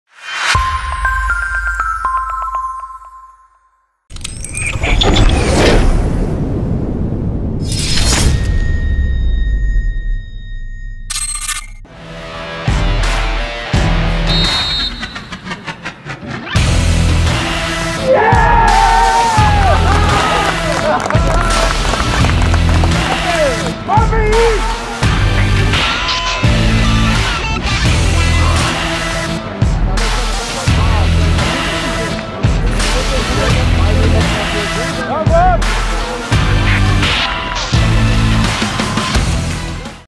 Grzegorz Janiczak - perfect free kick goal!!!
Rewelacyjne uderzenie Grześka w meczu ze starszymi kolegami z Wisły Kraków!